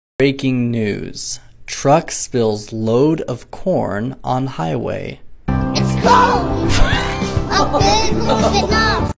BREAKING NEWS: Truck Spills Corn on Highway TikTok by dutchnovak
Funny repost tiktok about breaking news. The latest news is that corn spilled on the highway and it turned into a meme and not a repost on tiktok.